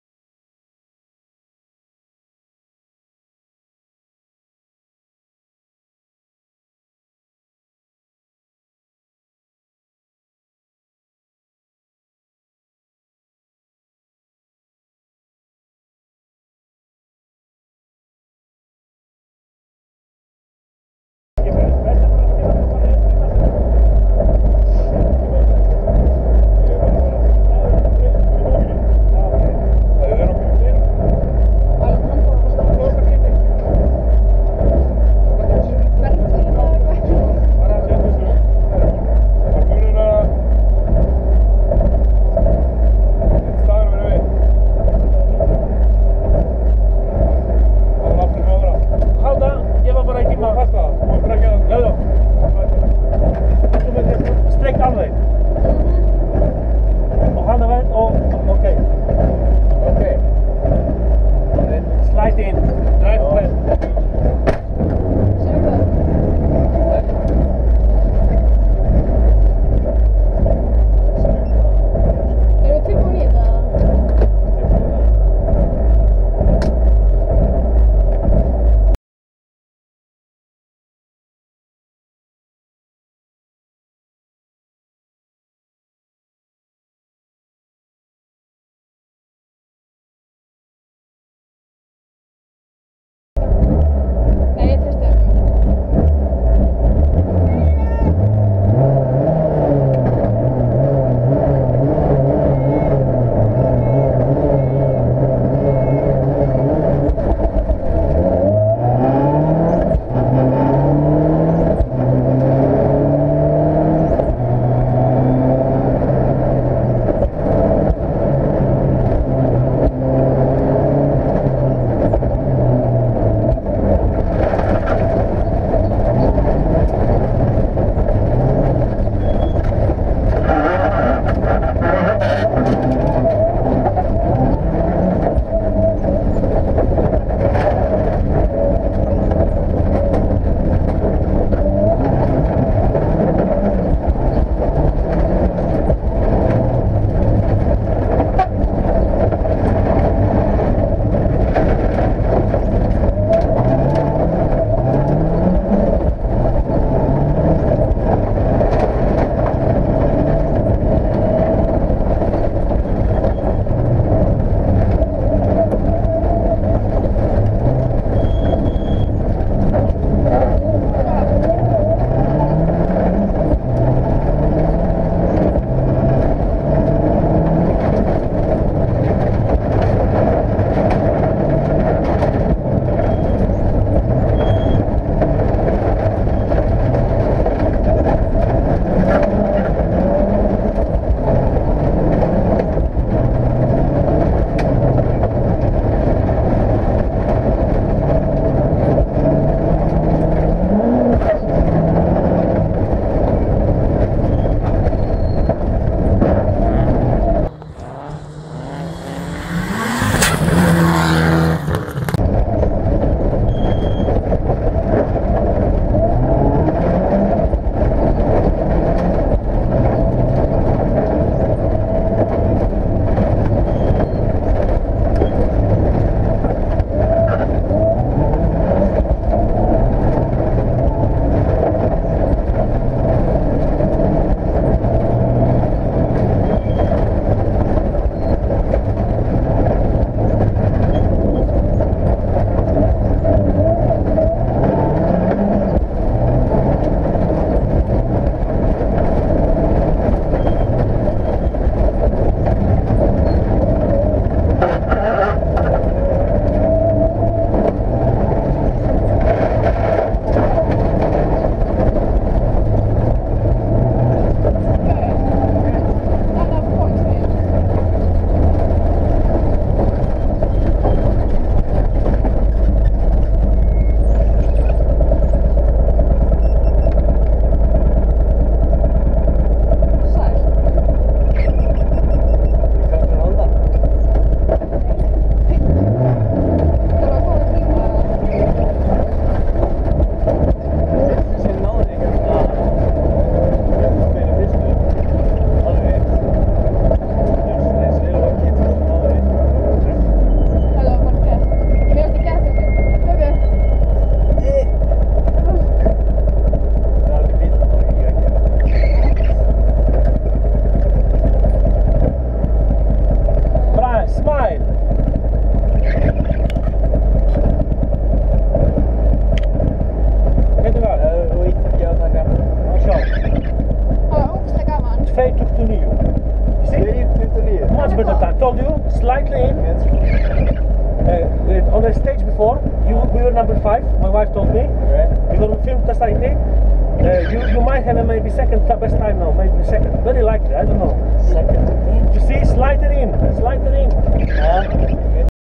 Gunnar karl reindi allt til að bæta tíman svo hann náði ástu eins og danni
rally reykjavík